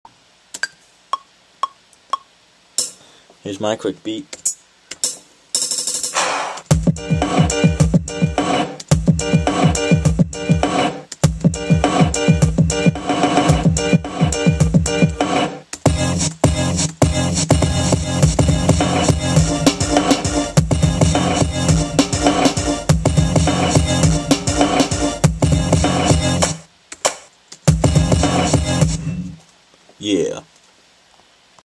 Ron Winter drum beat
Check out my beat that I made with the Ron Winter drums online... see what you can make